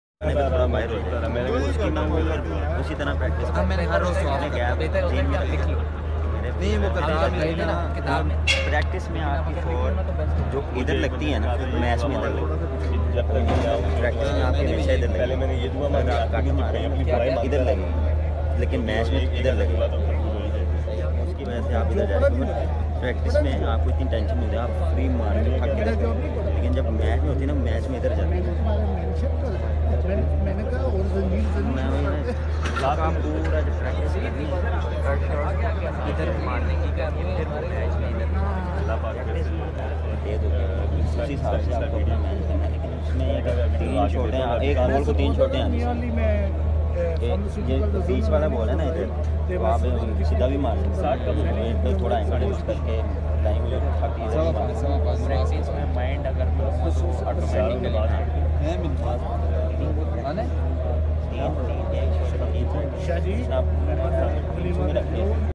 Babar Azam with Pakistani Youngster Rohail Nazir
Babar Azam giving advice to Rohail Nazir
Best video of PSL Draft ❤